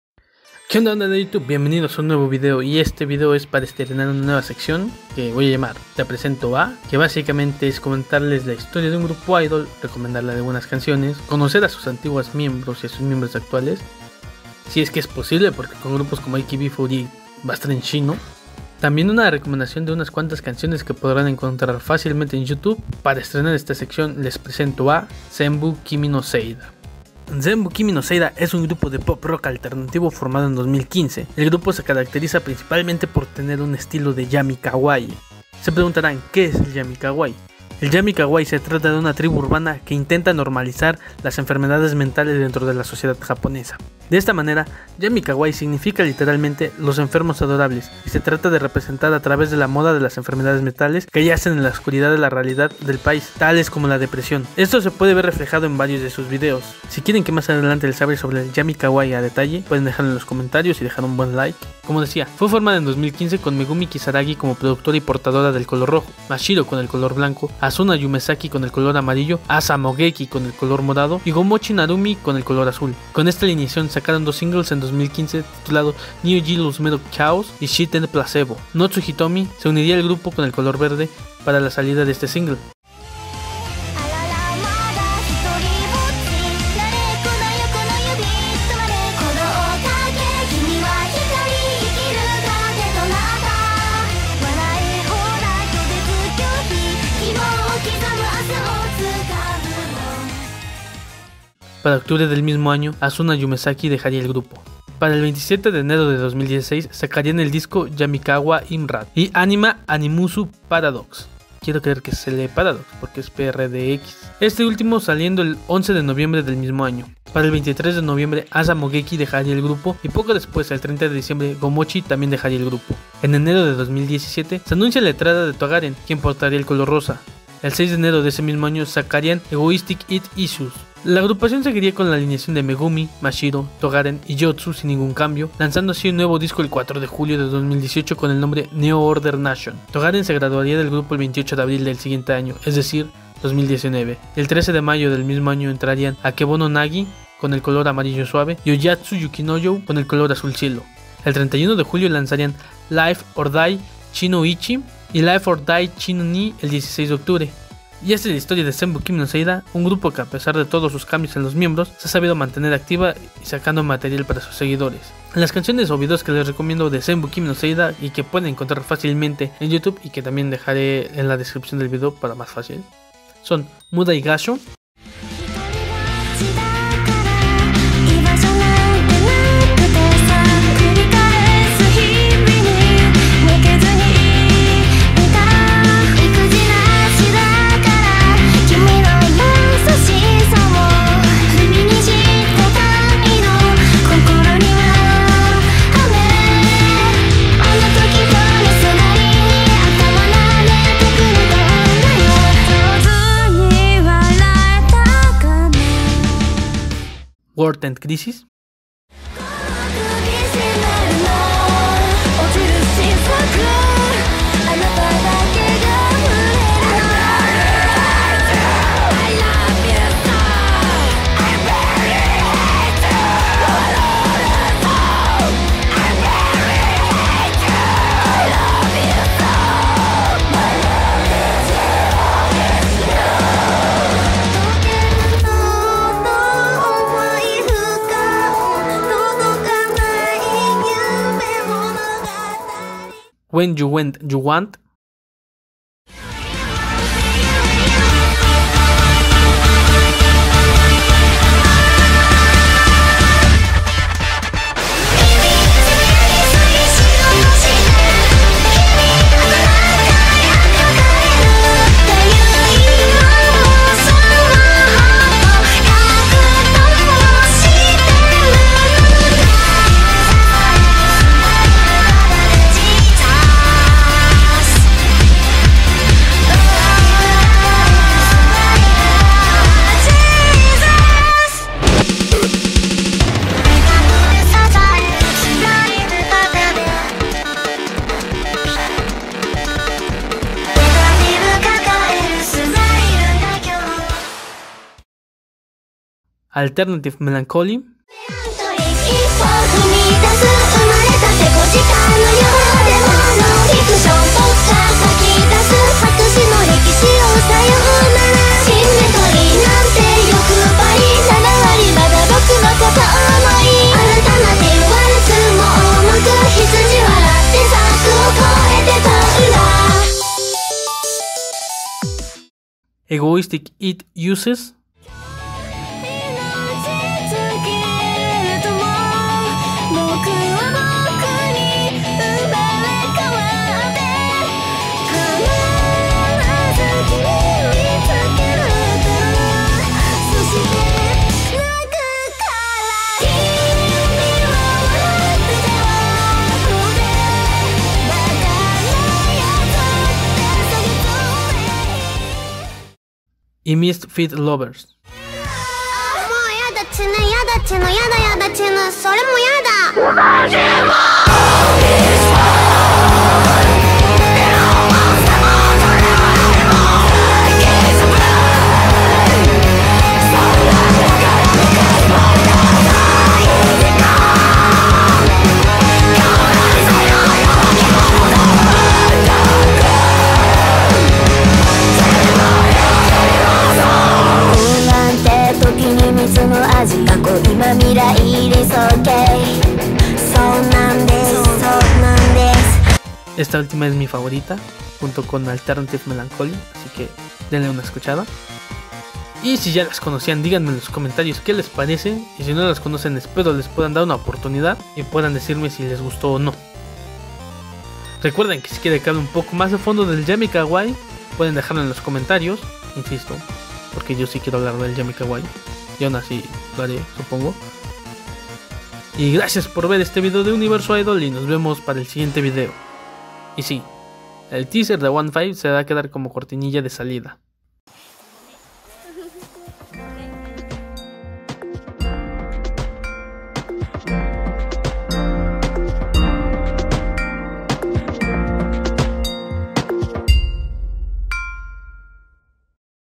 TE PRESENTO A ZENBU KIMI NO SEI DA
Hola gente de Youtube en ésta ocasión les traigo una pequeña introducción a uno de los grupos Idols que mas sigo y es Zenbu Kimi No Sei Da uwu.

como dije en el video les dejo los links de las canciones que les recomiendo uwur